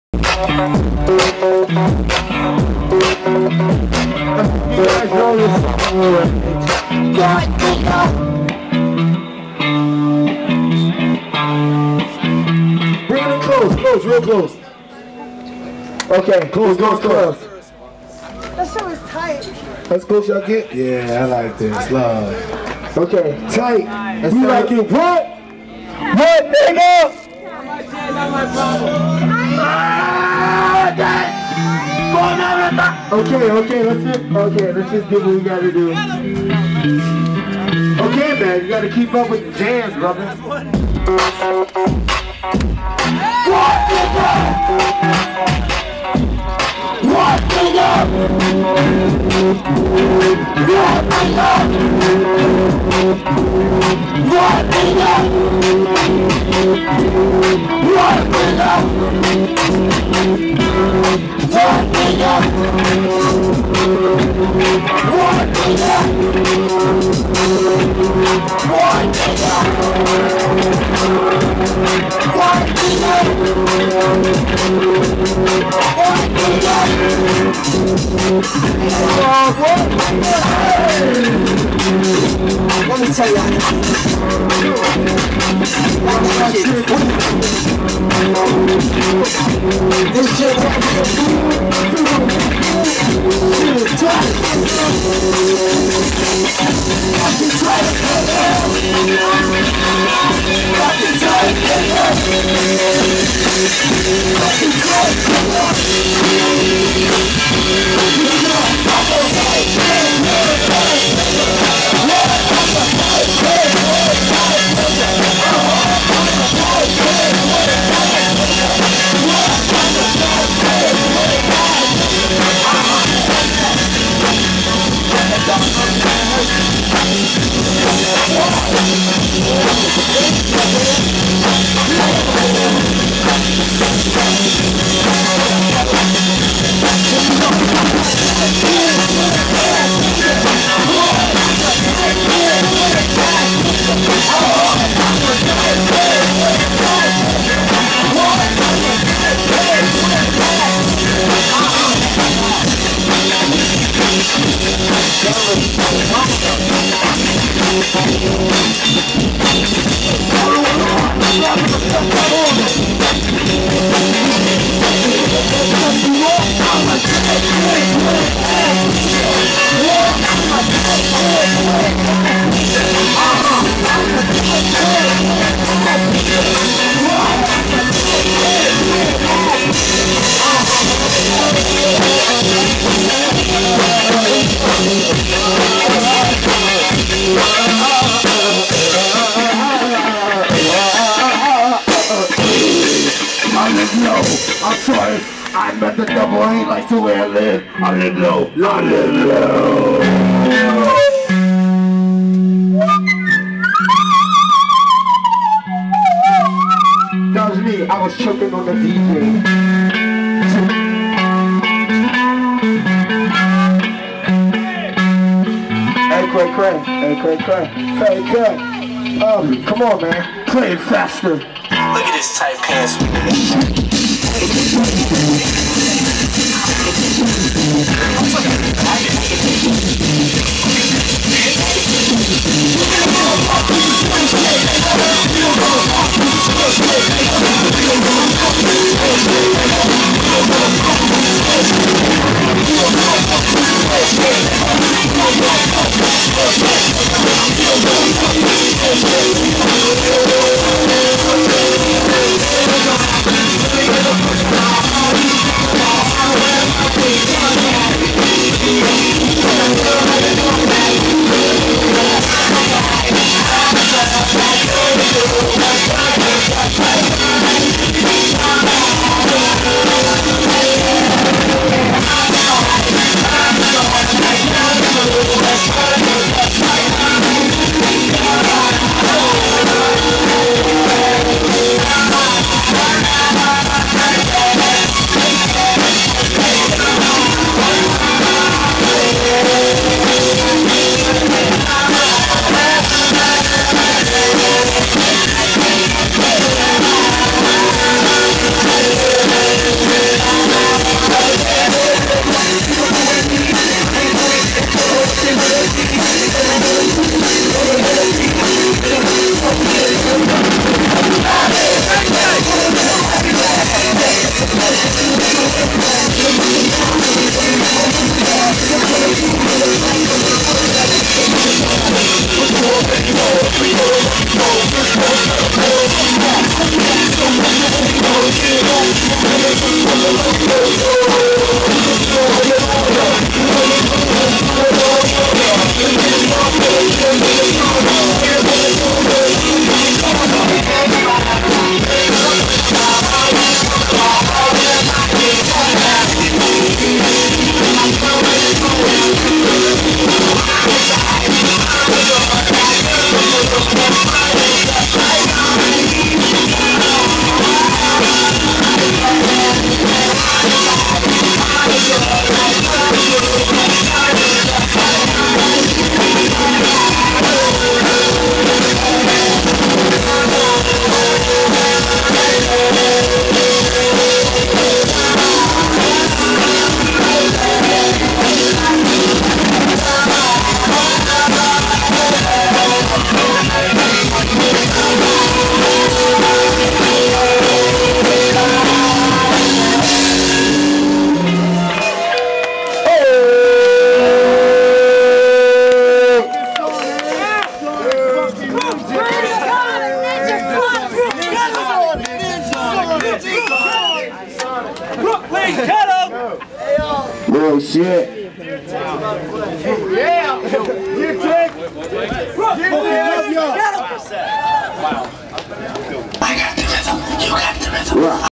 NINJASONIK @ SXSW 2008 - Secret LateNight Todd P Show
NINJASONIK @ SXSW 2008 - Secret Late-Night Todd P Show with Diplo and Blaqstarr.  Austin, TX  03/16/08